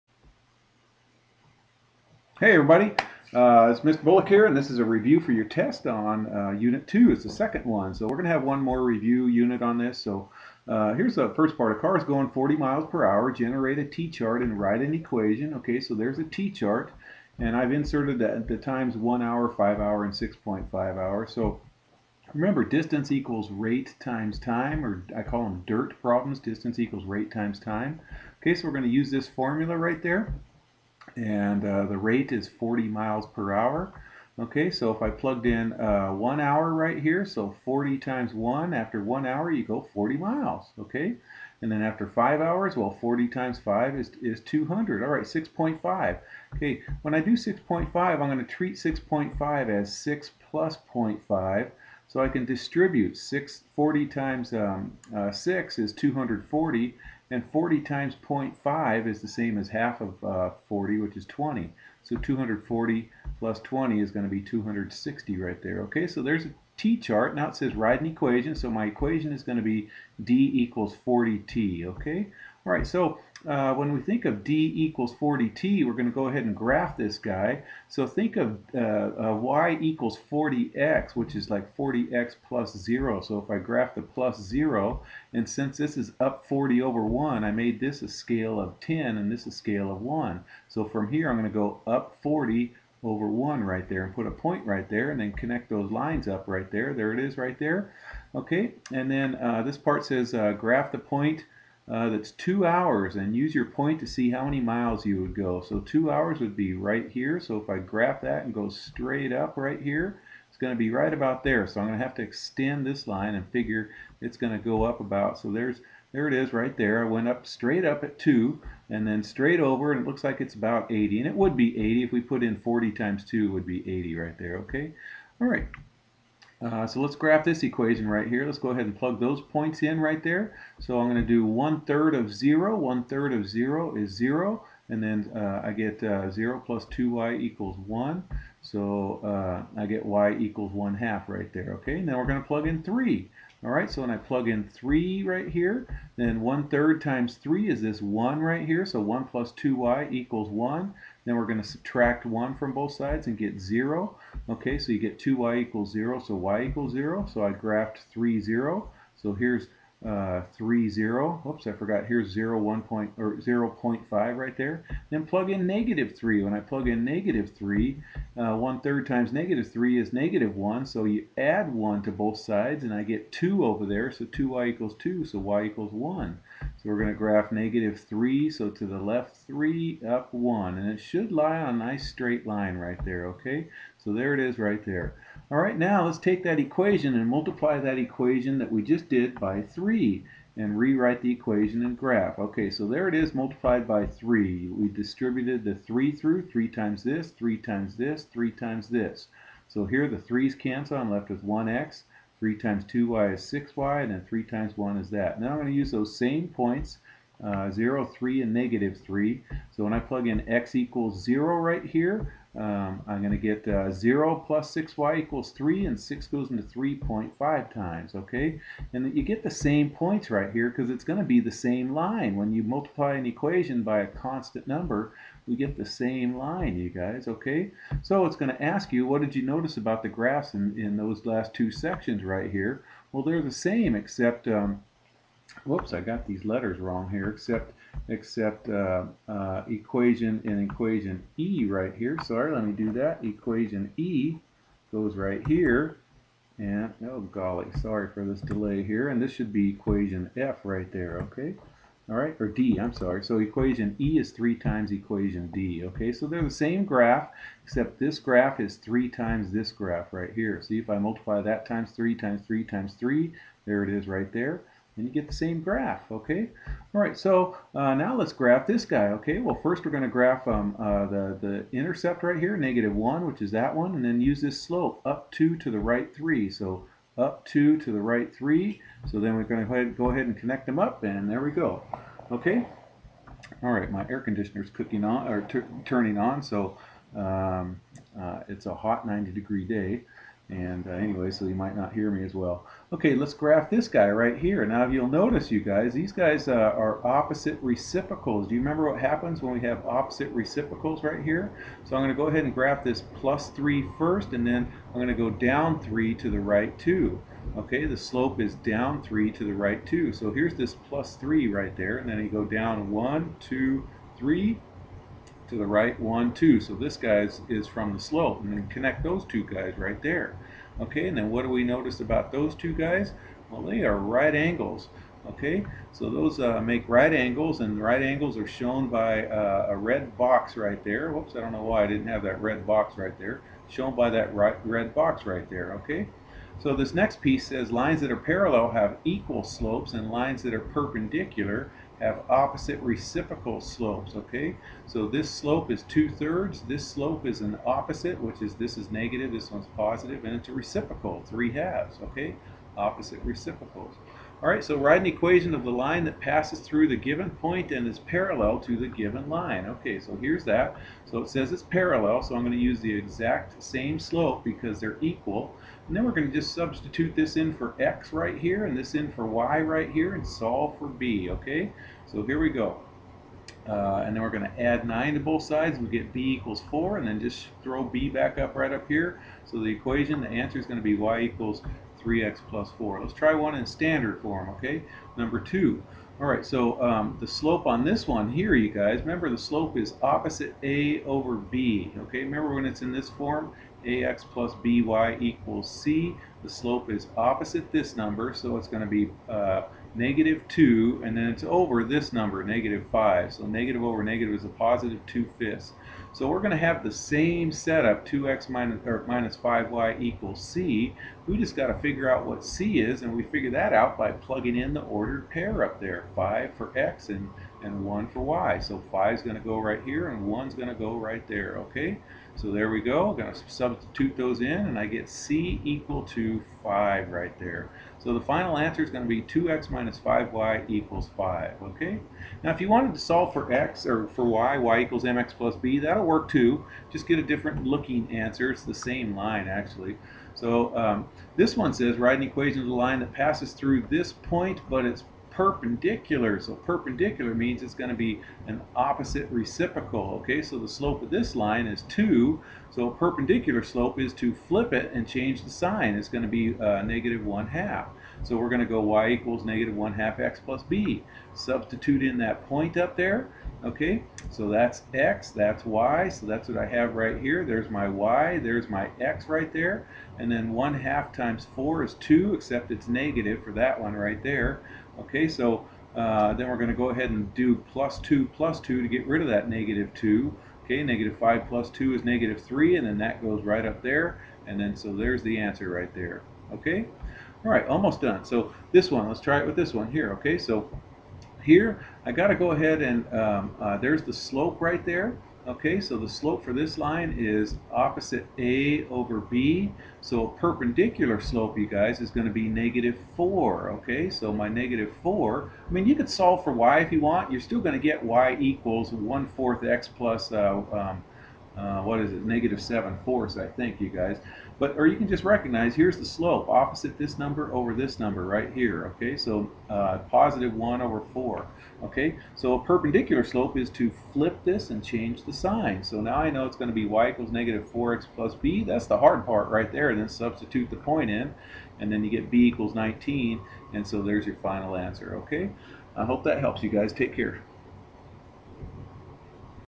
Review For Test on Unit 2 (Part 2)
This review is in 3 parts, with this video being the second one. This covers parallel and perpendicular lines, and writing equations of them. You can do this!!